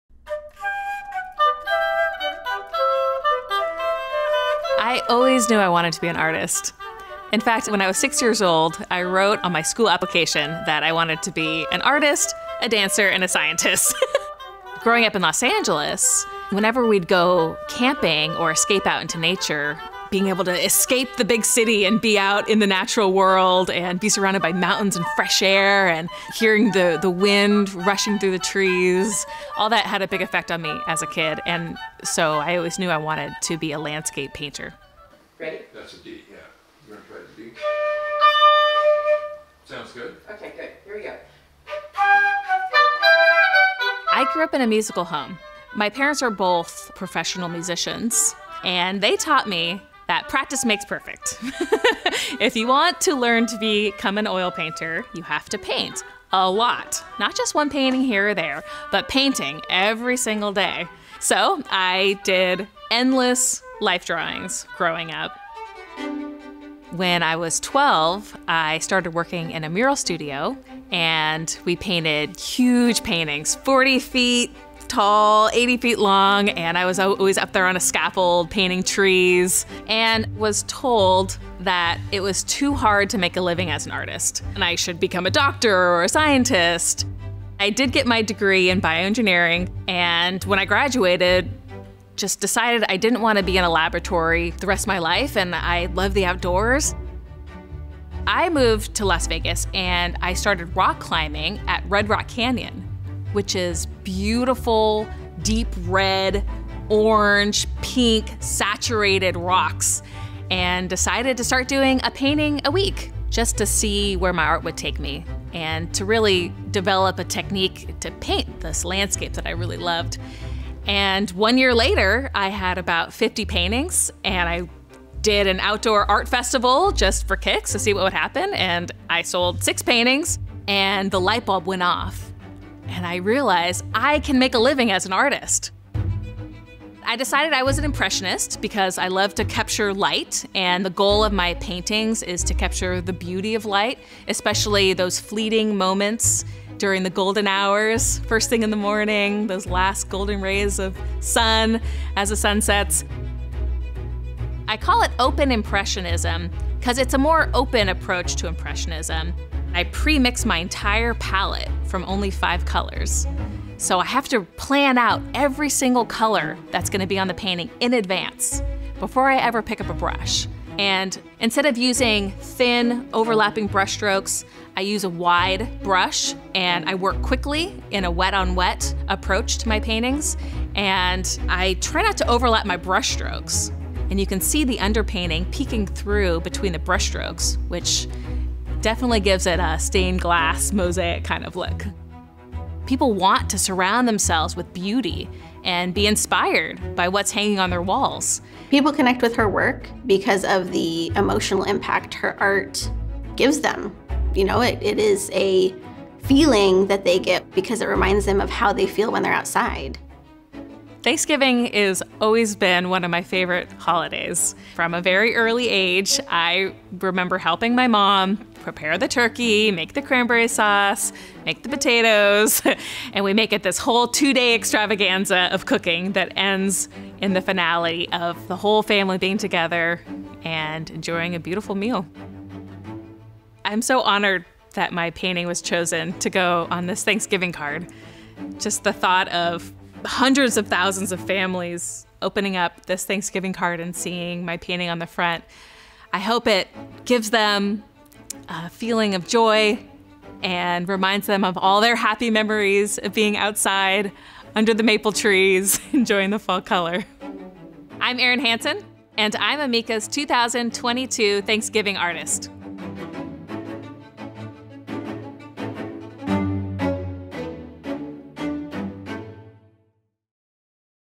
Introducing Amica’s 2022 Thanksgiving card artist
Meet the incredible artist behind the cover of the 2022 Amica Thanksgiving card, Erin Hanson! We’re proud to feature her painting “Maple Lights,” and you can view the card